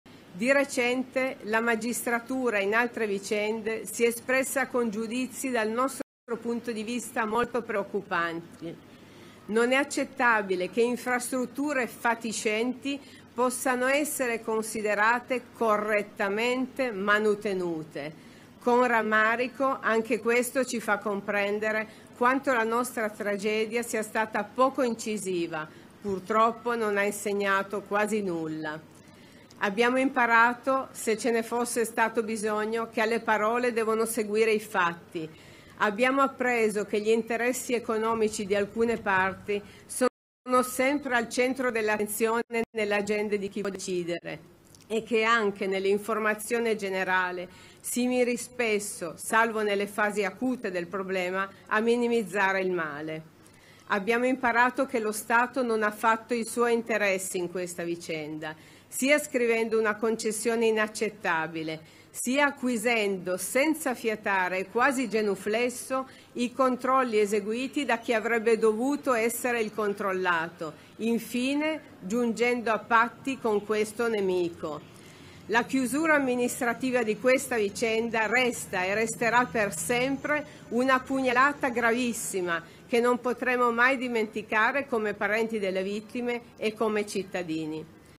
Ponte Morandi, comitato vittime: «Stato non ha fatto suoi interessi, sceso a patti con il nemico»
«Abbiamo imparato che lo Stato non ha fatto i suoi interessi in questa vicenda, sia scrivendo una convezione inaccettabile sia acquisendo senza fiatare, quasi genuflesso, i controlli eseguiti da chi avrebbe dovuto essere il controllato Infine giungendo a patti con questo nemico». Lo ha detto Egle Possetti presidente del comitato per le vittime durante la cerimonia in memoria delle 43 vittime del crollo del Ponte Morandi, avvenuto 5 anni fa. «La chiusura amministrativa di questa vicenda resta una pugnalata gravissima che non potremo mai dimenticare come parenti delle vittime e come cittadini».